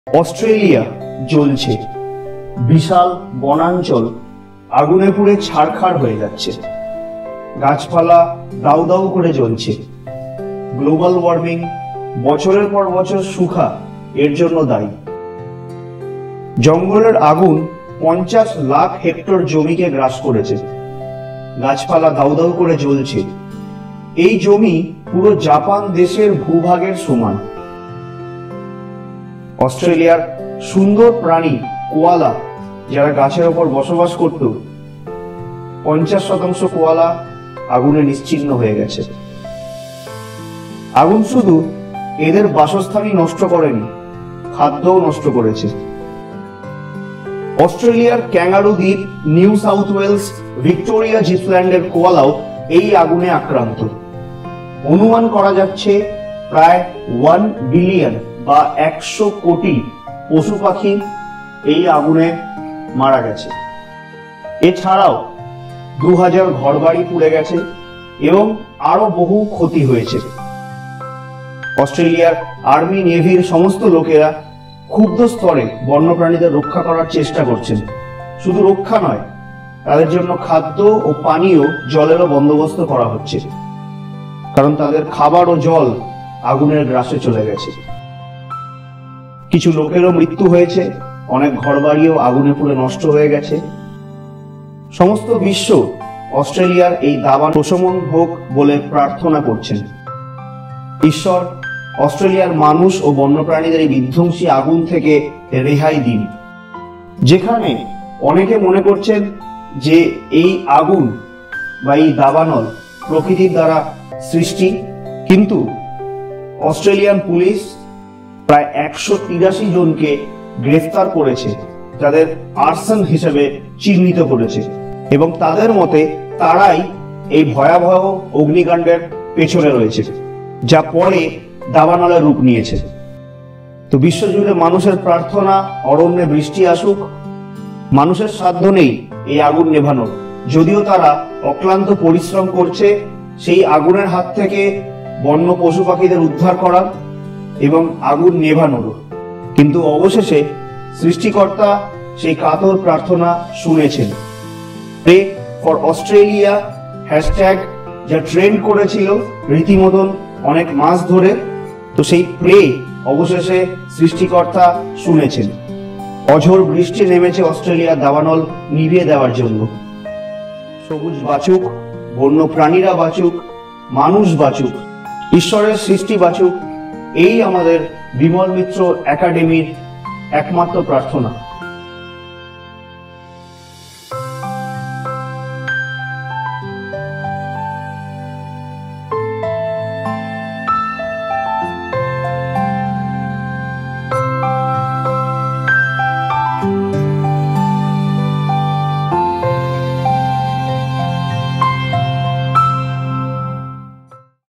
Pray For Australia | Australian Forest Fire & Massive Loss of Birds, Animals & Trees
This is a sad video and also our prayers for Australian Forest Fire & Massive Loss of Birds, Animals & Trees.
Our Salutes & Prayers for Australians in their valiant efforts to fight National Calamity.

Australia forest fire pashto new video
australia bushfires
australia fire animals
australian animals
australian forest fire
fires in australia 2020